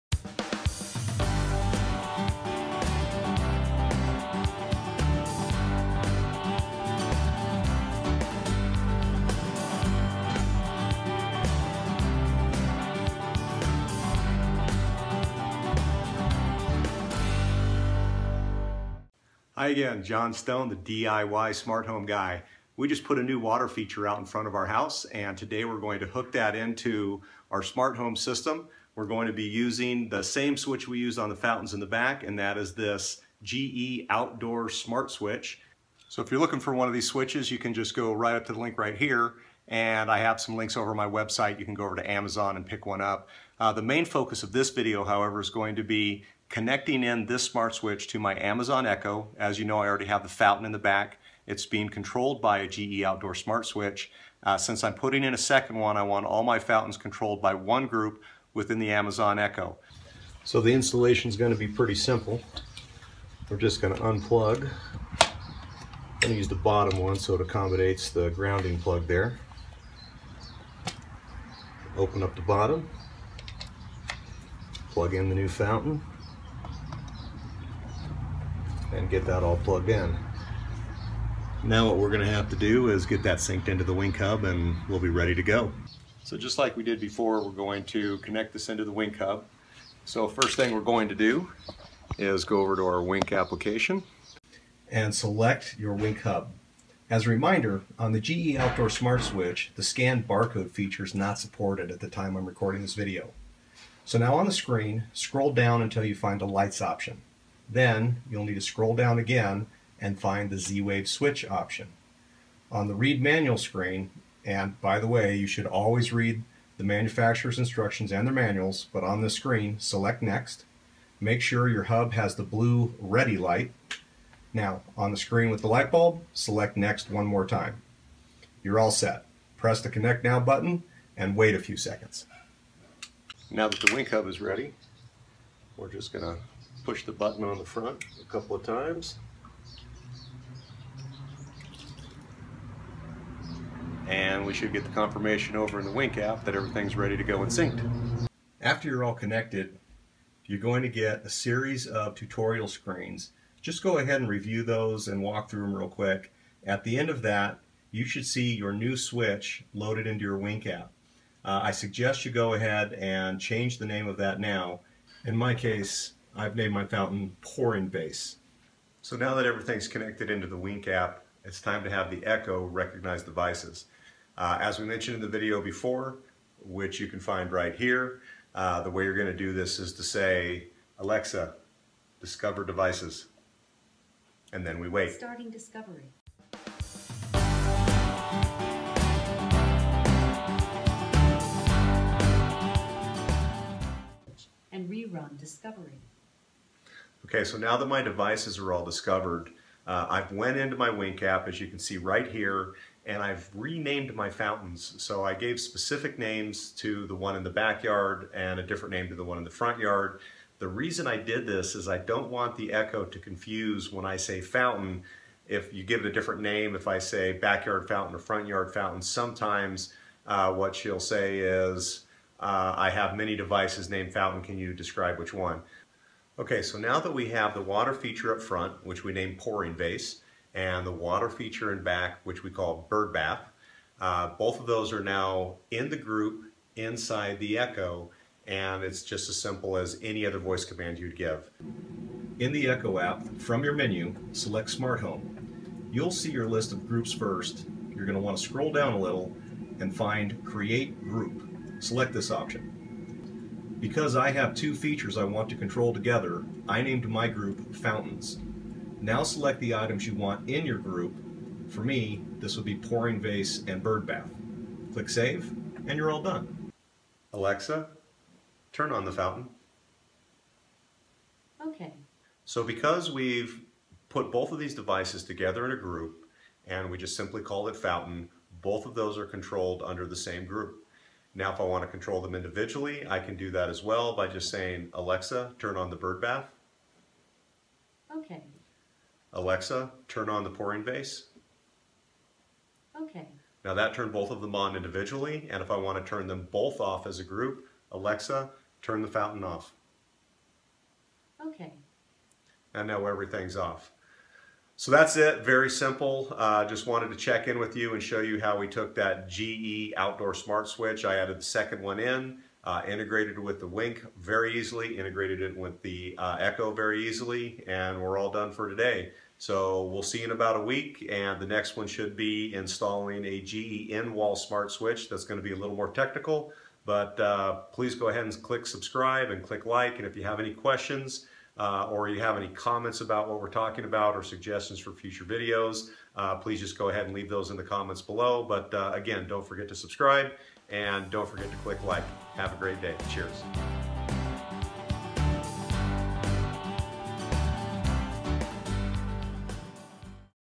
Connect and Setup GE Outdoor Smart Switch to Wink Hub and Amazon Echo for Smart Home Automation
The DIY Smart Home Guy is adding a new water feature. In this episode we plan to hook this into our Wink smart home system and then into our Amazon Echo. To do this we will add a second GE Outdoor Zwave Smart Switch (GE 12720). We already have a fountain in the back yard. Our goal is to allow for independent control of each water feature as well as group control via the Echo.

Products in this video:

GE 12720

Wink Hub

Amazon Echo (Alexa)

This video provides step by step instructions for installing the smart switch, connecting it to the Wink hub and then into the Amazon Echo. Since the Wink does not support the scan barcode setup for this GE Switch, we will show you the simple way to connect the device with screen by screen instructions.

You will see how to have Alexa discover devices and even receive tips on naming devices to ensure that the Echo does not get confused when you are issuing voice commands. Once your devices are discovered, we walk through how to create the Echo group to control the devices as a pair. An essential piece in controlling both smart switches together.